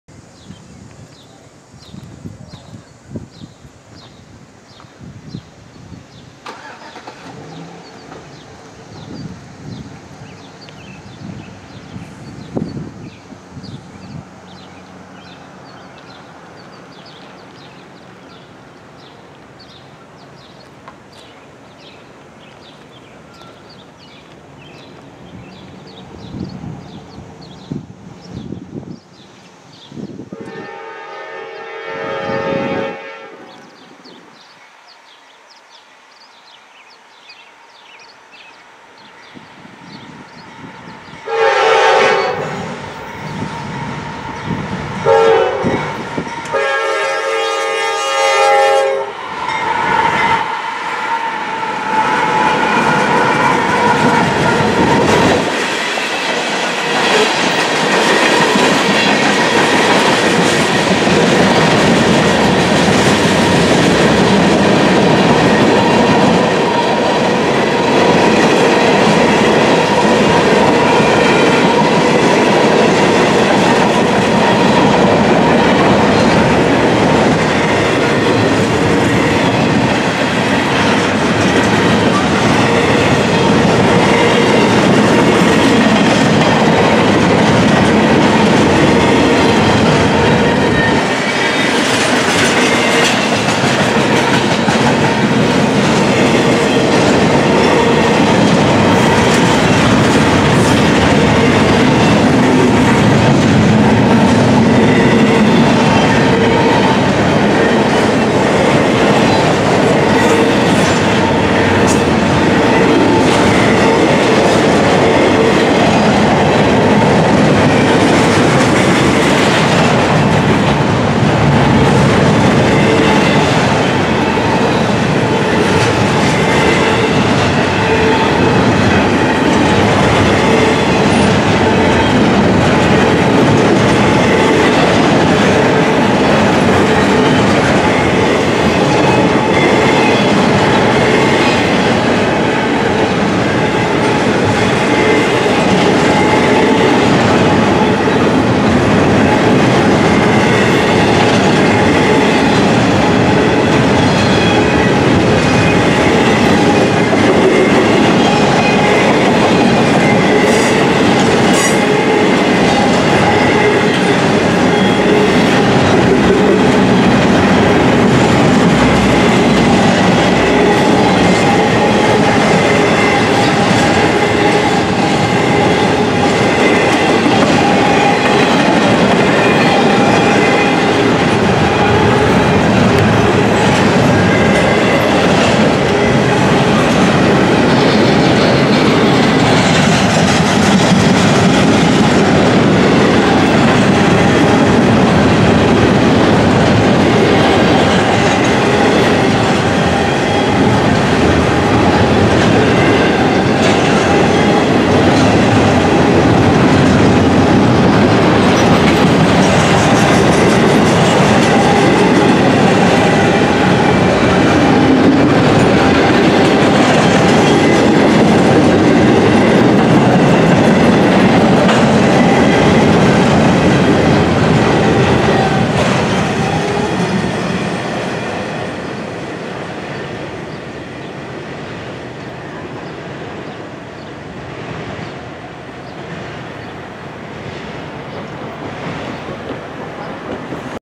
NORFOLK SOUTHERN GE AC44C6M Northbound Manifest Mix/Intermodal Container Train
A northbound Mix/Intermodal Container Lead by Norfolk Southern GE AC44C6M #4351 Engine passing through at Milepost Rosemont.